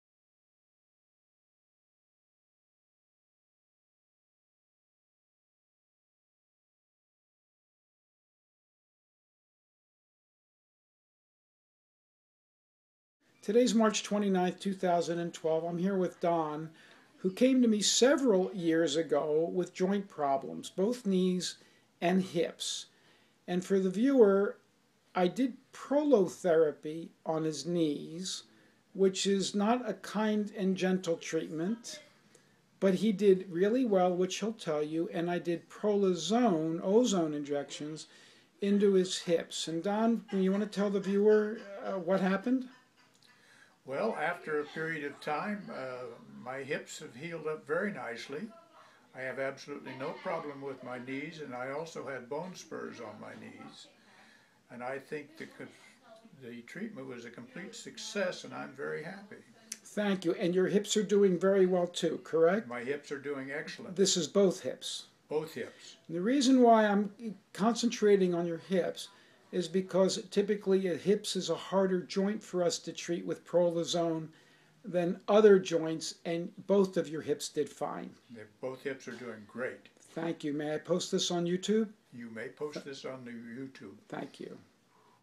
Both hips fixed with Prolozone in little time
Hip arthritis fixed with Prolozone.
Knees fixed with standard prolotherapy.